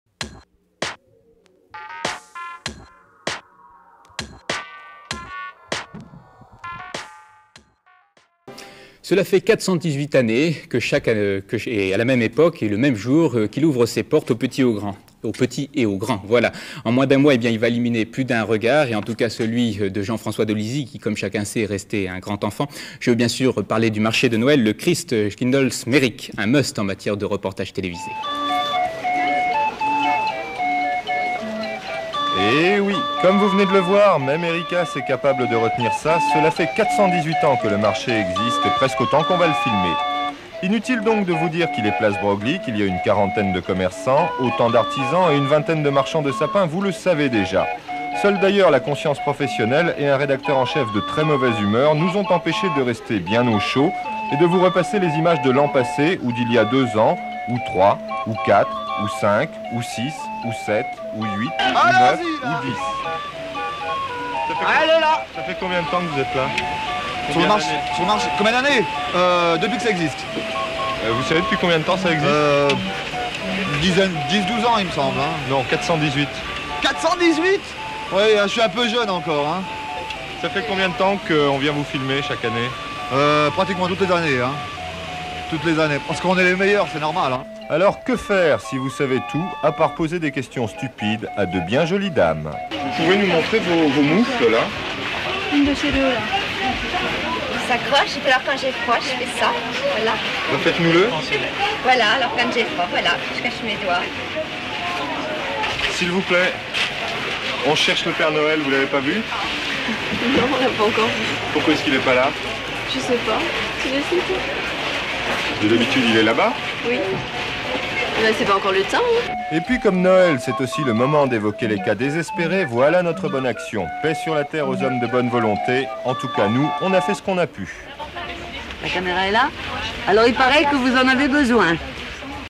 Culte : quand un journaliste en a marre des marchés de Noël | Archive INA
28/11/1987 reportage Jean-François  Dolisi & Denis Becker,
Quelques images du célèbre "Christkindelsmarik" qui se tient tous les ans pendant la période de l'avent place BROGLIE à Strasbourg, Ce marché de Noël où les Strasbourgeois viennent acheter jouets, friandises, décorations mais aussi le traditionnel sapin. Il existe depuis 418 années et ouvre ses portes aux petits et aux grands.